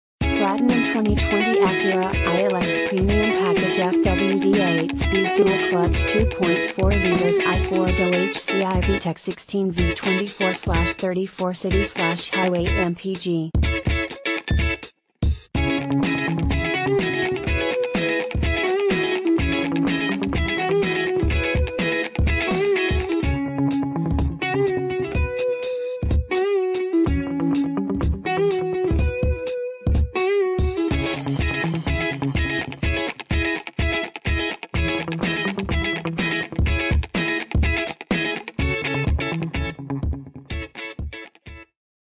2020 Acura ILX 20156100
2020 Acura ILX Premium Package

Acura of Pembroke Pines
15601 Pines Blvd.

Platinum 2020 Acura ILX Premium Package FWD 8-Speed Dual-Clutch 2.4L I4 DOHC i-VTEC 16Vbrbr24/34 City/Highway MPG